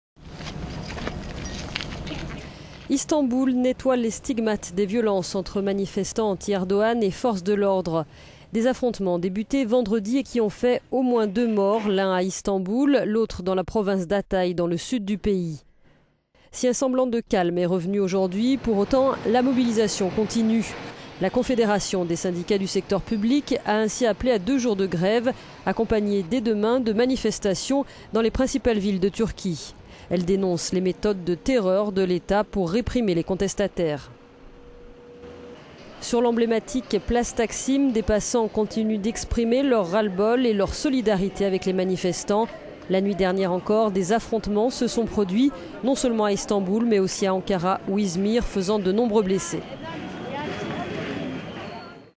Retour au calme à Istanbul mais la mobilisation continue
Istanbul nettoie les stigmates des violences entre manifestants anti-Erdogan et forces de l'ordre. Des affrontements débutés vendredi et qui ont fait au moins deux morts, l'un à Istanbul, l'autre dans la province d'Hatay dans le sud du pays. Si un semblant de calme est revenu ce mardi, la mobilisation continue. La confédération des syndicats du secteur public (KESK) a ainsi appelé à deux jours de grève, accompagnés dès demain de manifestations dans les principales villes du pays. Elle dénonce les méthodes de "terreur de l'Etat" pour réprimer les contestataires.

Sur l'emblématique place Taksim, des passants continuent d'exprimer leur ras-le-bol et leur solidarité avec les manifestants. La nuit dernière encore, des affrontements se sont produits non seulement à Istanbul mais aussi à Ankara ou Izmir, faisant de nombreux blessés.